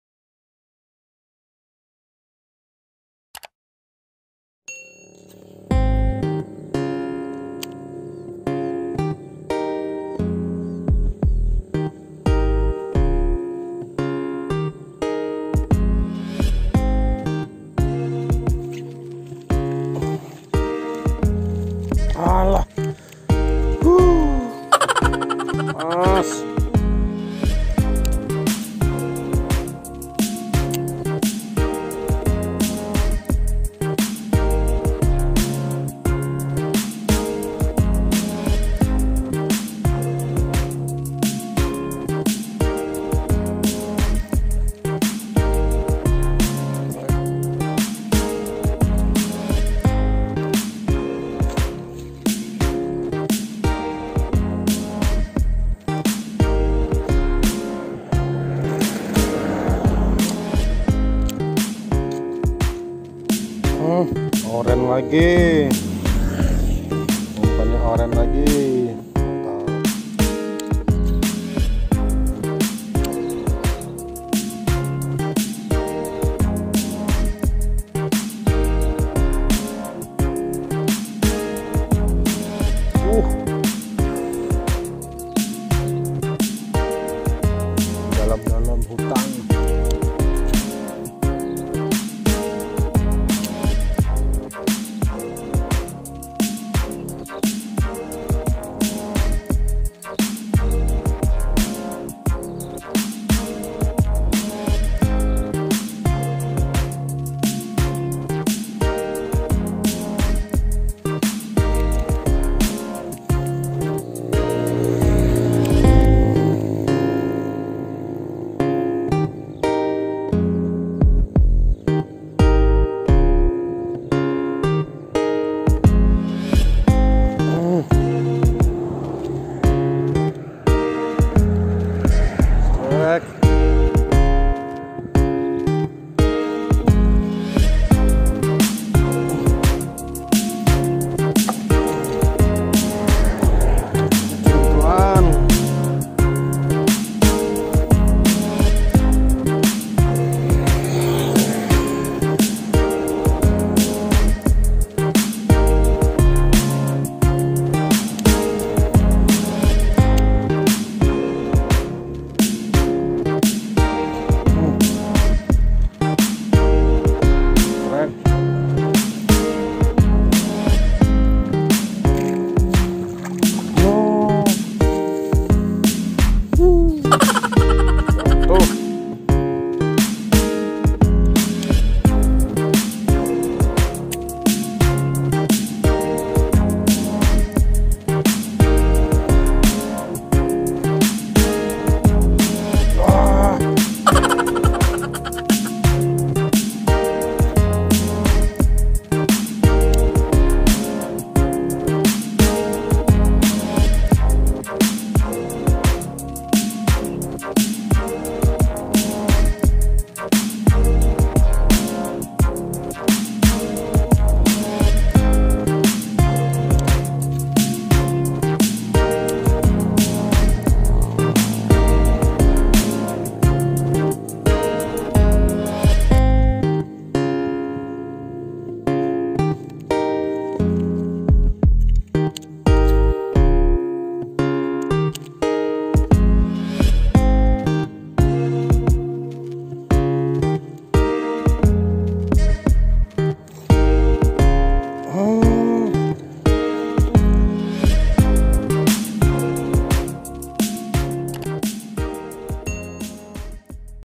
Casting gabus sore hari, Pinggir jalan raya #60
Sore hari casting gabus di pinggir jalan Lintas kabupaten. Hasilnya lumayan lah..